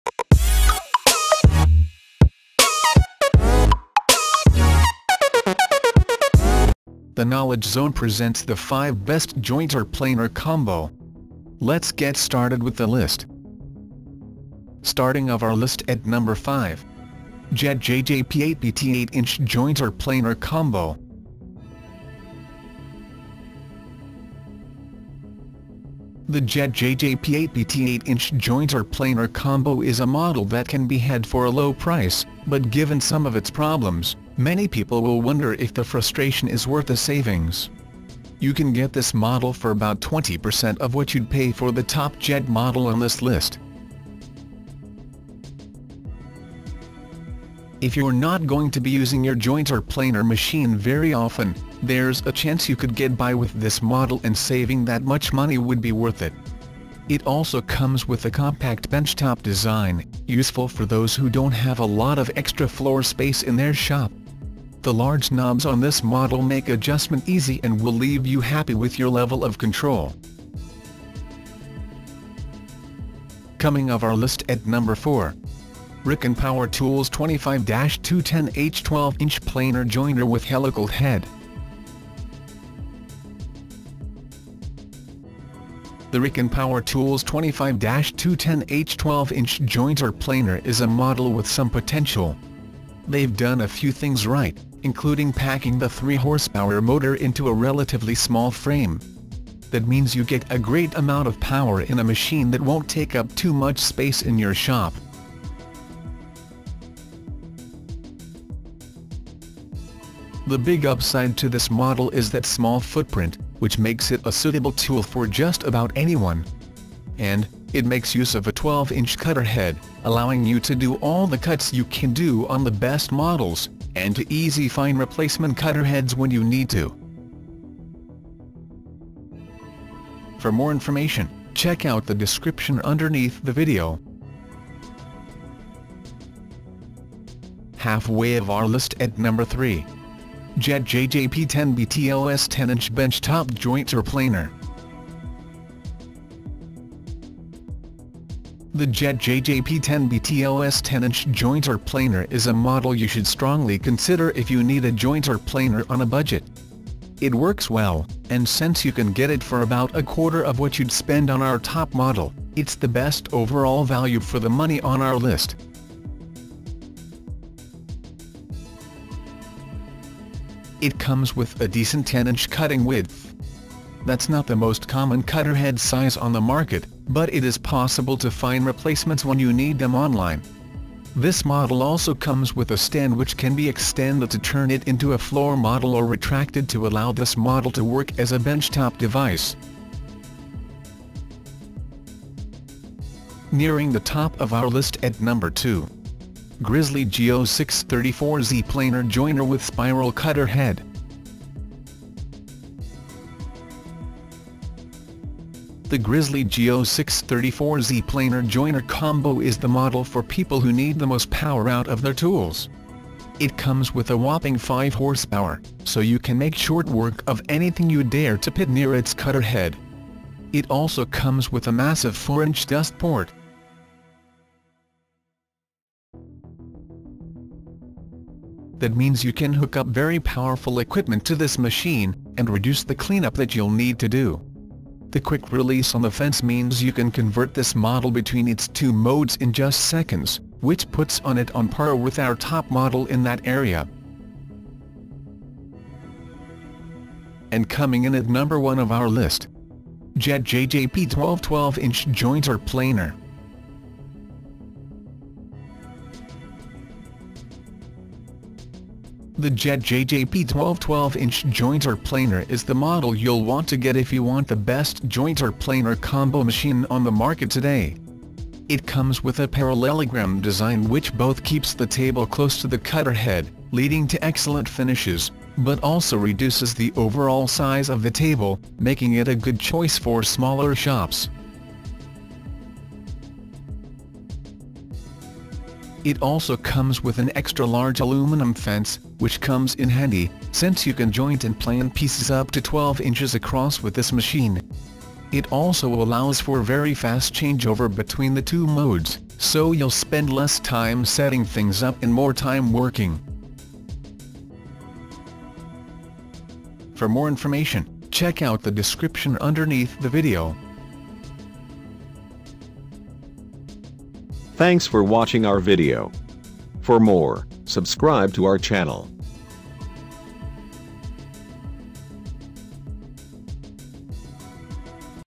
Best Jointer Planer Combo 2021
Hello,
Looking for the detailed review of Best Jointer Planer Combo? We have tried to show the details in this Review. You can decide which Jointer you should buy and which one is good for you, After reading this exclusive review.

Here is the list of 5 Best Jointer Planer Combo.

01. JET JJP-12 12-Inch Jointer Planer

02. Grizzly G0634Z Planer/Jointer with Spiral Cutter head

03. Jet JJP-10BTOS 10-Inch Bench-Top Jointer/Planer

04. RIKON Power Tools 25-210H 12-Inch Planer/Jointer with Helical Head

05. Jet JJP-8BT 8" Jointer/Planer Combo

Thanks!